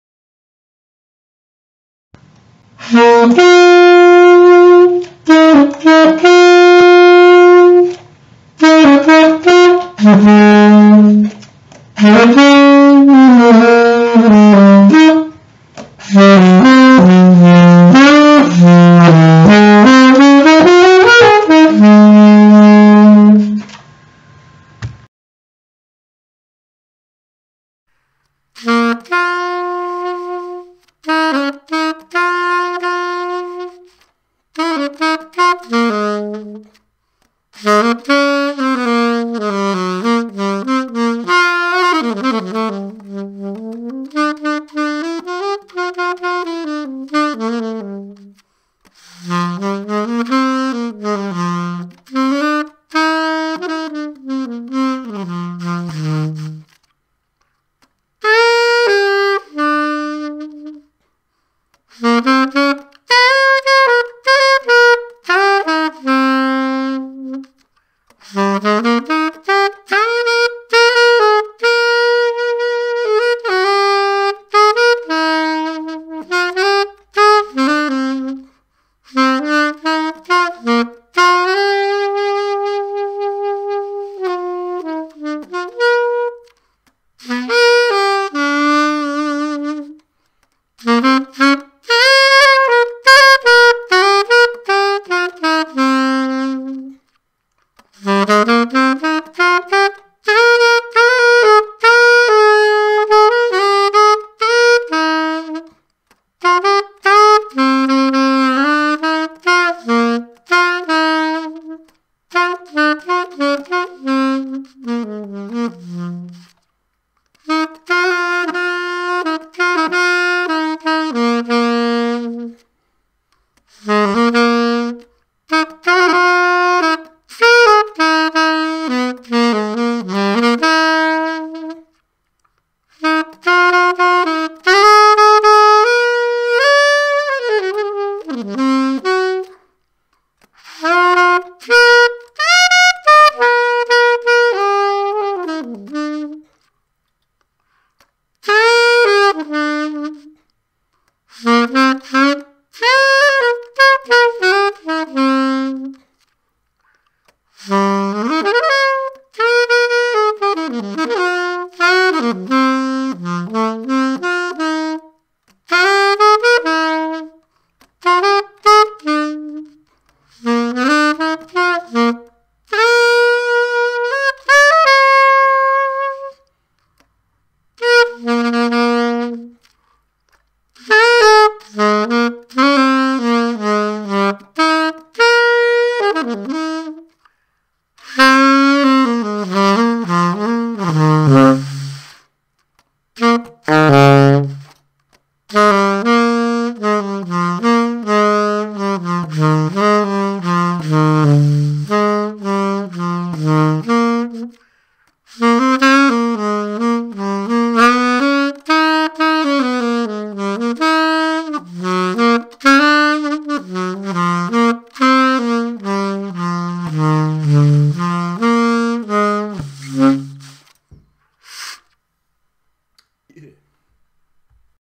Blue Snowball Test
Just testing out my Blue Snowball condenser mic and comparing it to my laptop's internal mic.
Massive difference in saxophone sound quality.

(I'm a tad bit rusty on Alto. More of a Tenor sax man)